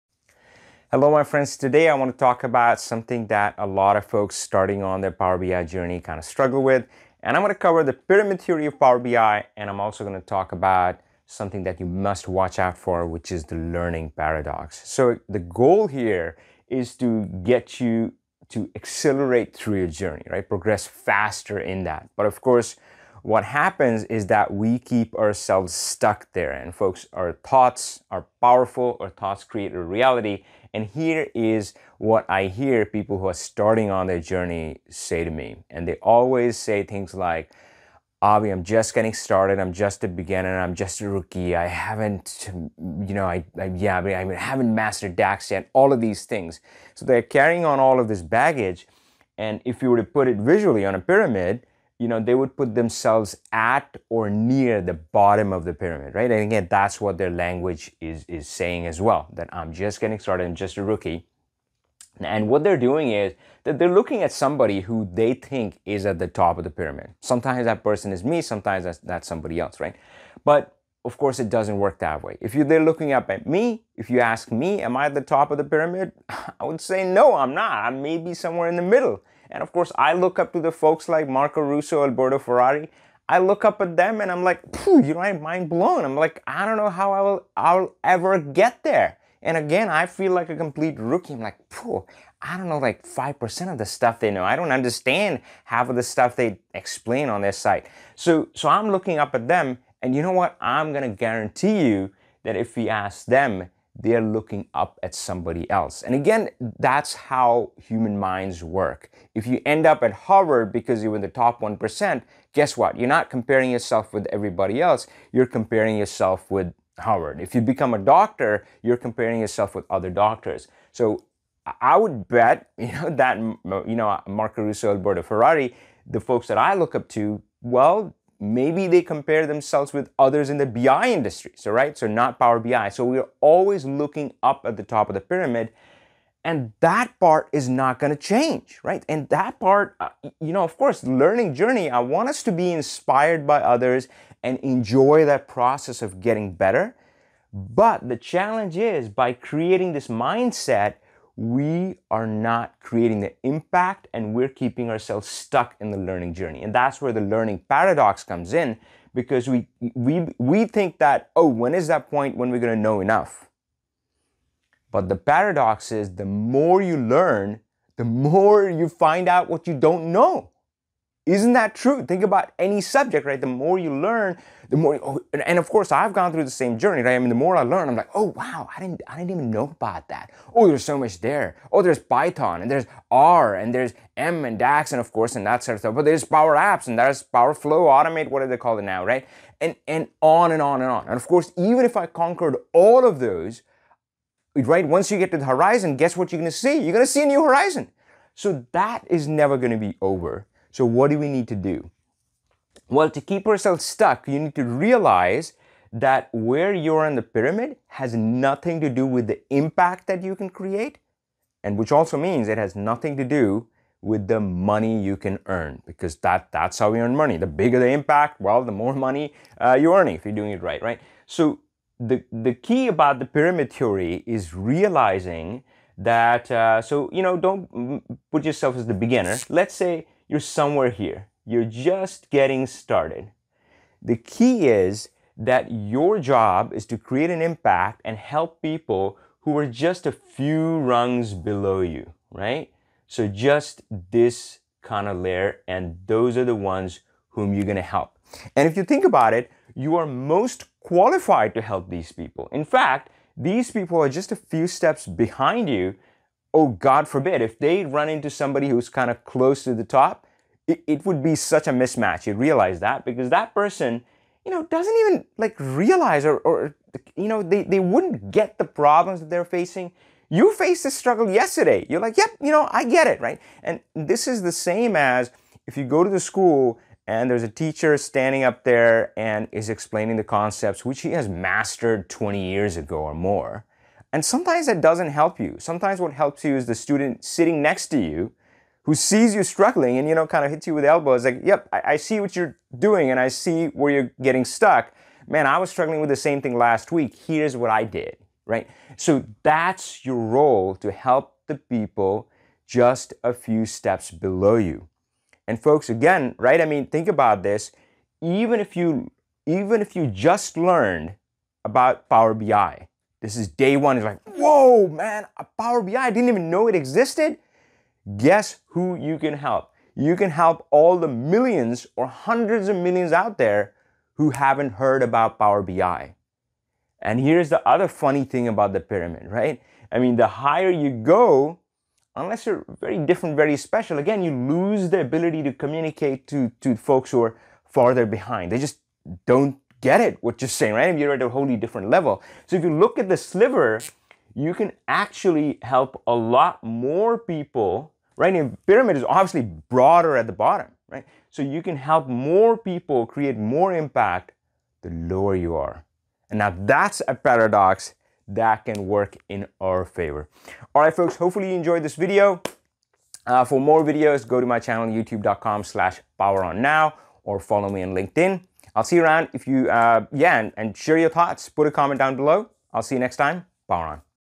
Stuck in Power BI Beginner Mode? Use ★Pyramid Theory★ to Accelerate 🚀 Your Results (& Impact)
Tired of being stuck in the Power BI Beginner Mode? Learn how you can use the ★Pyramid Theory★ to break out of rookie mode. Accelerate your results and impact.